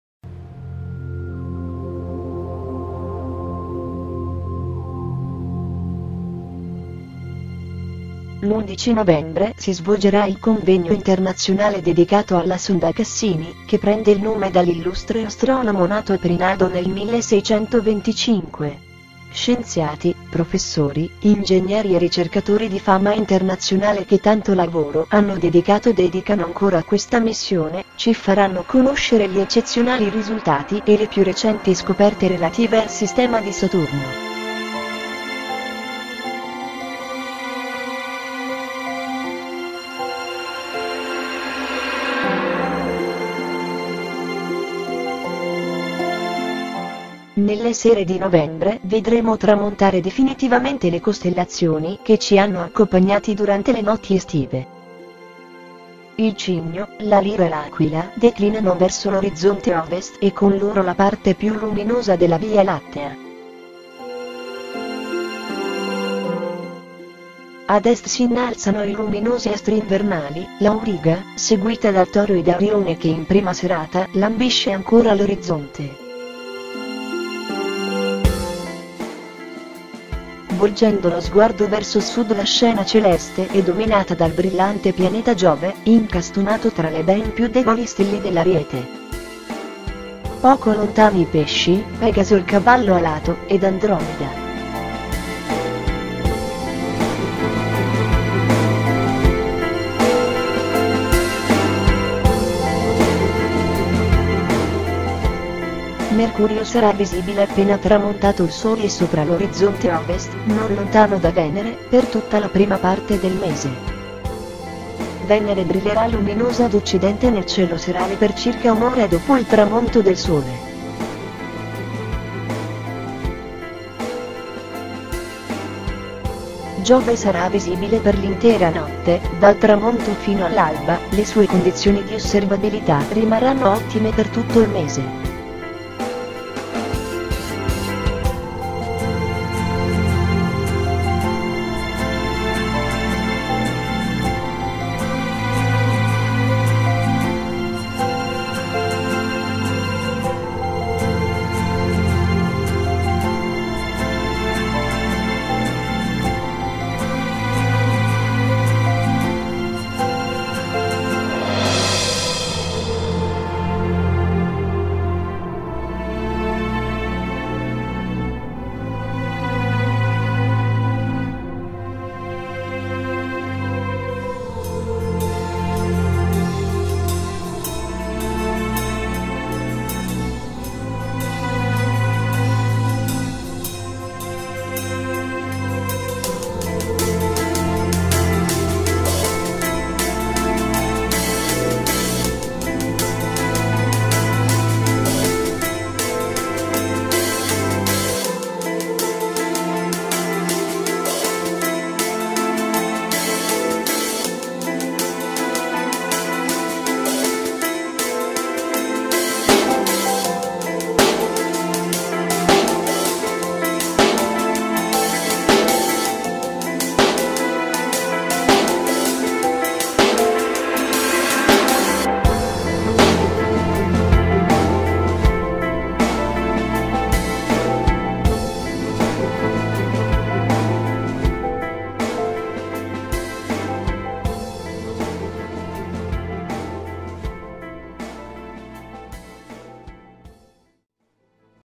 Il Cielo di Novembre 2011, eventi celesti, costellazioni e pianeti visibili.
Convegno Cassini.
L'undici Novembre 2011 si svolgerà il Convegno Internazionale dedicato alla sonda Cassini, che prende il nome dall'illustre astronomo nato a Perinaldo nel 1625.
Scienziati, professori, ingegneri e ricercatori di fama internazionale che tanto lavoro hanno dedicato e dedicano ancora a questa Missione,  ci faranno conoscere gli eccezionali risultati e le più recenti scoperte relative al sistema di Saturno.

COSTELLAZIONI A NOVEMBRE
Nelle sere di Novembre vedremo tramontare definitivamente le costellazioni che ci hanno accopagnati durante le notti estive.
Il Cigno, la Lira e l'Aquila declinano verso l'orizzonte OVEST e con loro la parte più luminosa della Via Lattea.

Ad Est si innalzano i luminosi astri invernali, l'Auriga, seguita dal Toro e da Orione che in prima serata lambisce ancora l'orizzonte.

Volgendo lo sguardo verso sud la scena celeste è dominata dal brillante pianeta Giove, incastonato tra le ben più deboli stelle dell'Ariete.
Poco lontani i Pesci, Pegaso il cavallo alato, ed Andromeda.

PIANETI

Mercurio sarà visibile appena tramontato il Sole sopra l'orizzonte Ovest, non lontano da Venere, per tutta la prima parte del mese.

Venere brillerà luminosa ad occidente nel cielo serale per circa un'ora dopo il tramonto del Sole.

Giove sarà visibile per l'intera notte, dal tramonto fino all'alba, le sue condizioni di osservabilità rimarranno ottime per tutto il mese.